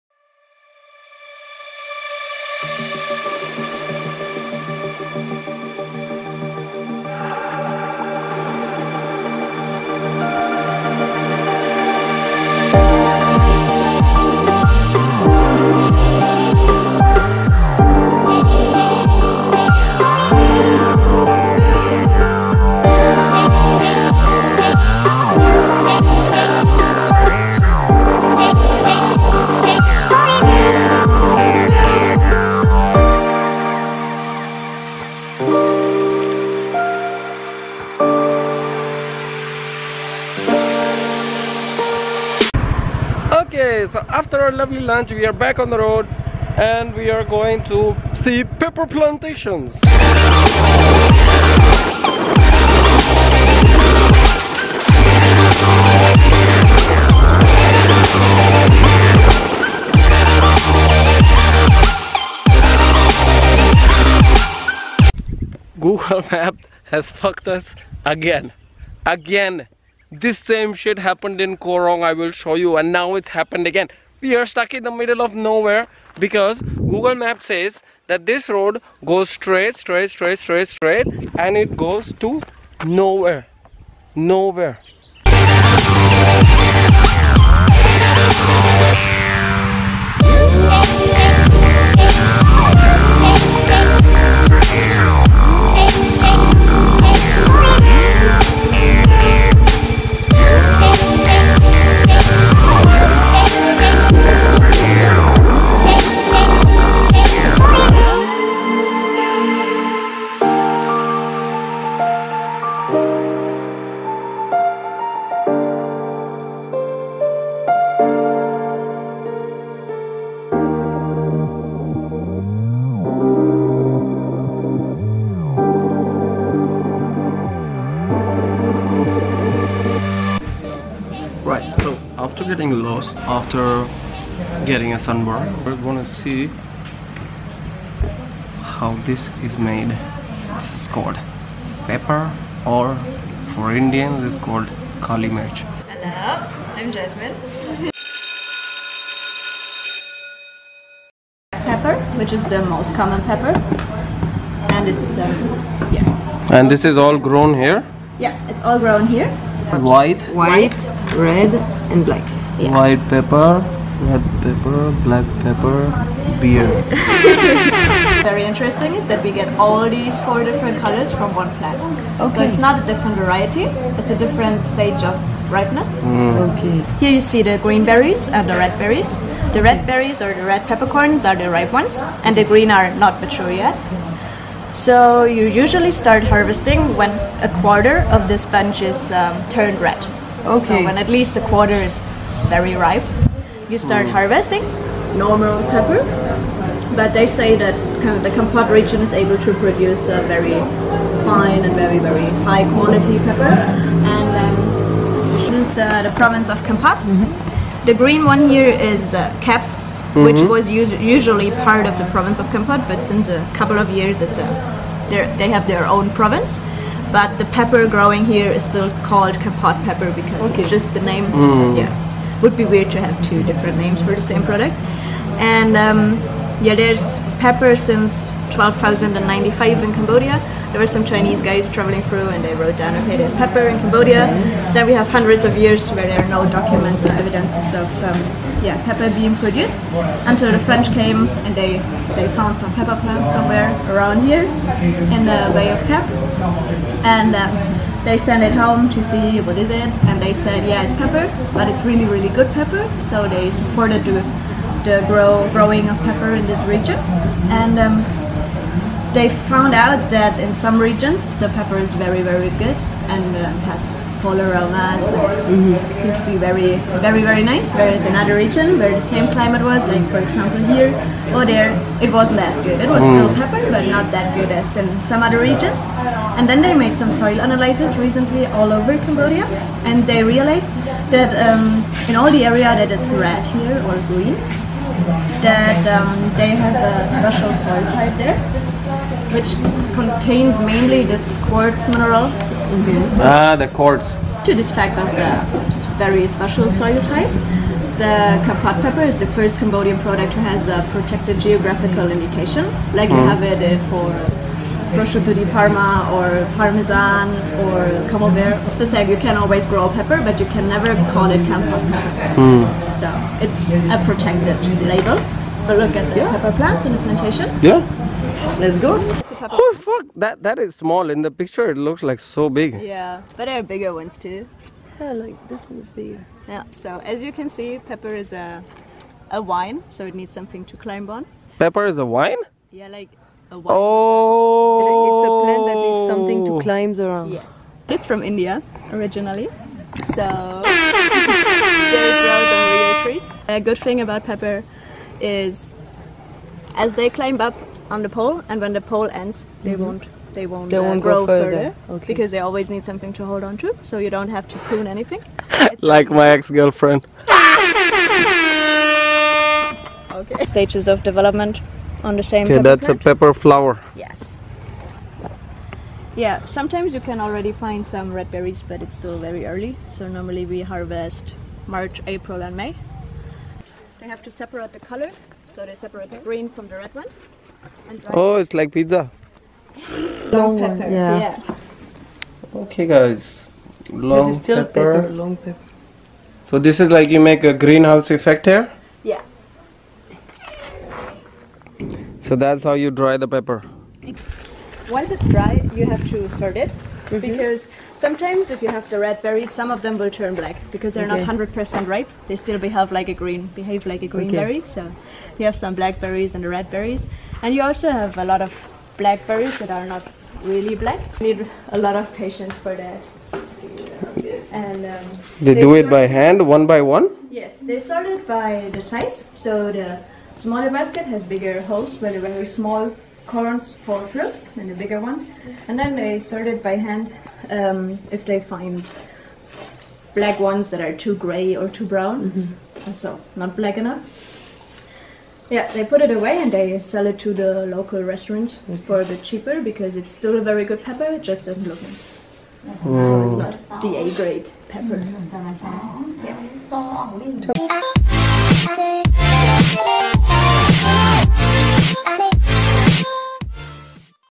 CAMBODIAN PEPPER : WHY ITS THE BEST IN THE WORLD?
Kampot pepper is very special. It grows best at the foot of the mountains because of the quartz in the soil," says Sorn Sothy, the owner of Sothy's Pepper Farm, a small pepper farm near Kampot, Cambodia. The region, along the country's southern coast, is famed for the spice, which was the first Cambodian product to receive a Protected Geographical Indication (the same certification that protects regional products like Champagne). The mineral-rich soil and rainy weather in the high-elevation areas near Kampot and Kep are perfect for pepper production, and I was here to see it firsthand.